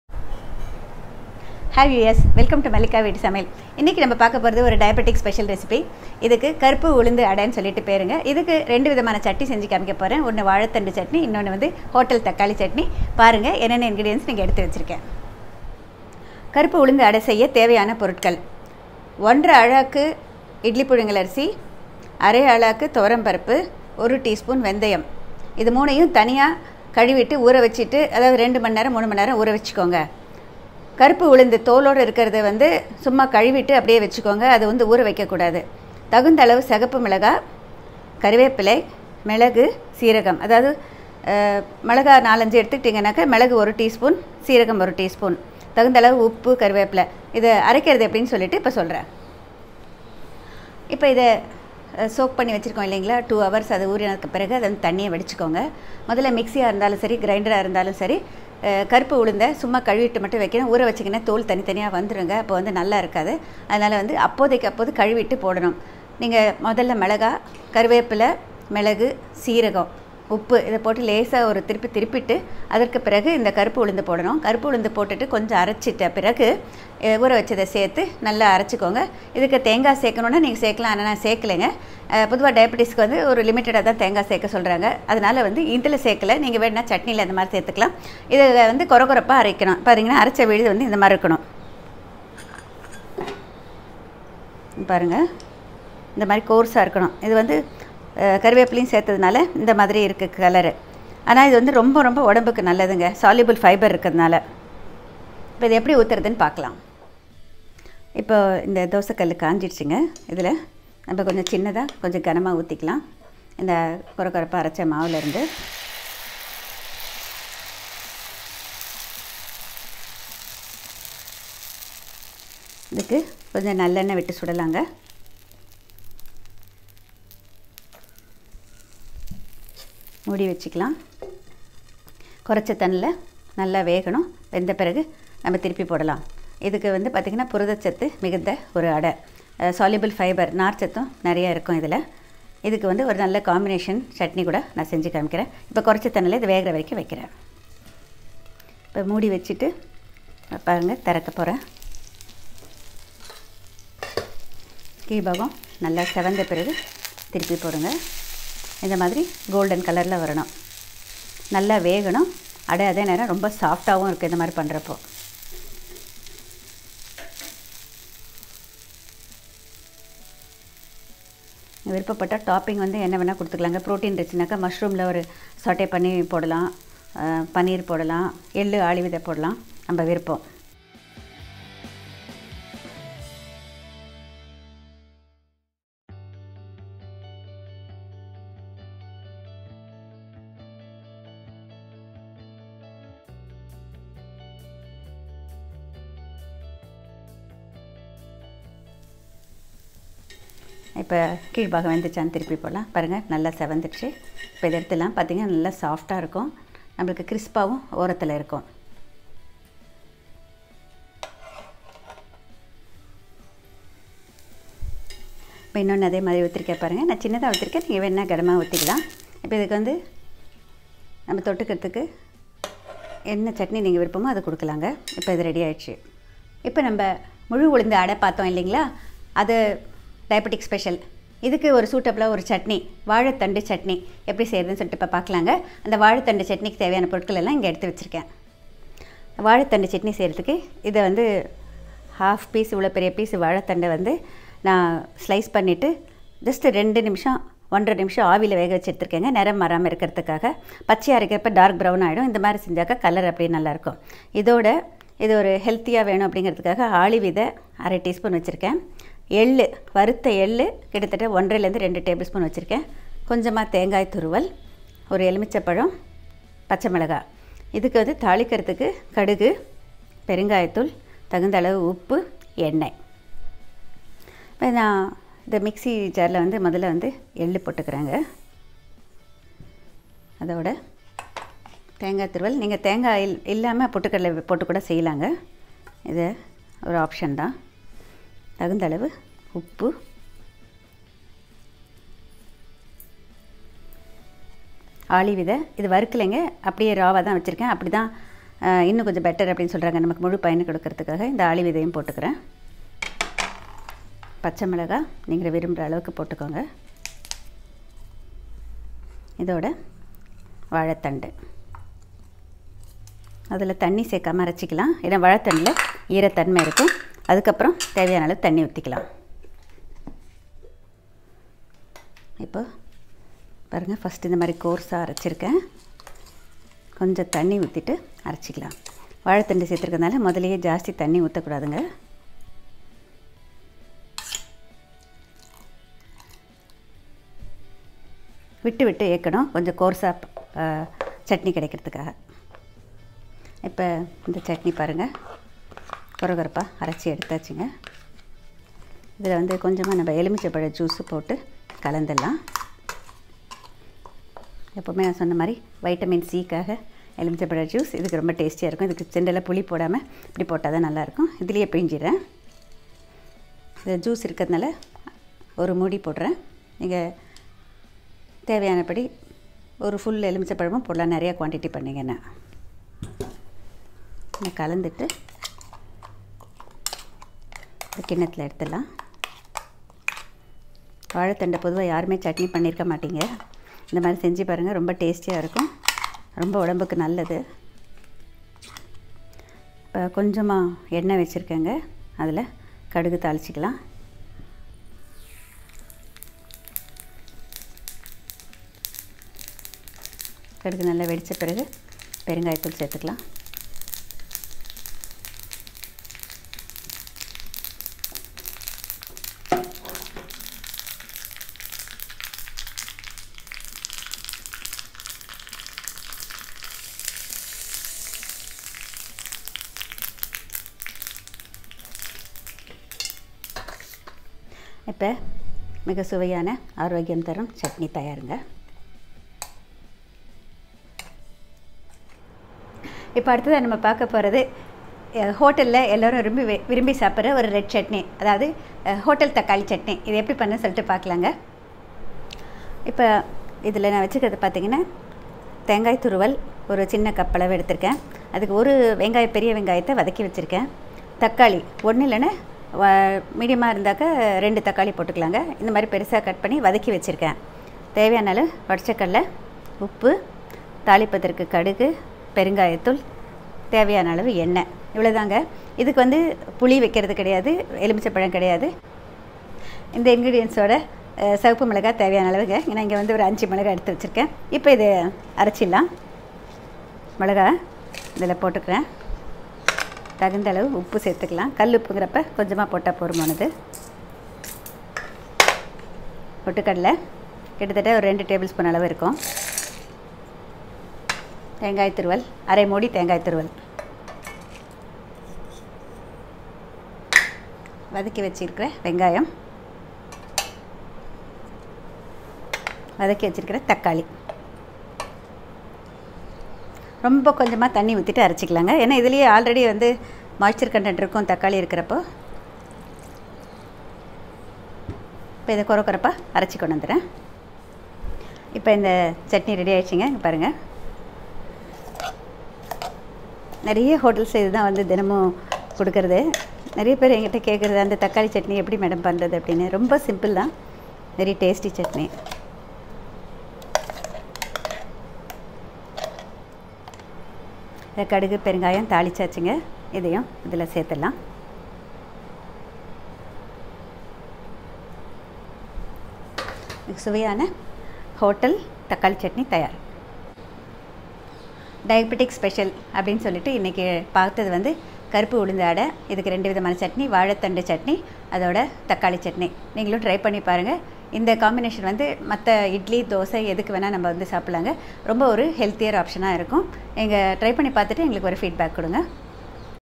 Diabetes Special Ulundhu adai / உளுந்து அடை, வாழைத்தண்டு சட்னி ,சிகப்பு சட்னி
Black gram adai : Black urad -1/2 cup , Idli Rice - 1 1/2 cups , Thuar Dhal -1/2 cup , pepper , cumin , red chillies, Curry leaves, salt, oil
Plantain stem Chutney : Vazhai Thandu sliced and steamed , sesame seeds , flax seeds ,green chillies ,coconut,salt,lemon
Red Chutney ( Hotel style ) / Tomato Chutney : 1 large onion diced and fried, 1 large tomato diced and fried , red chillies, coconut, salt, oil,Roasted gram, mustard seeds, Asafoetida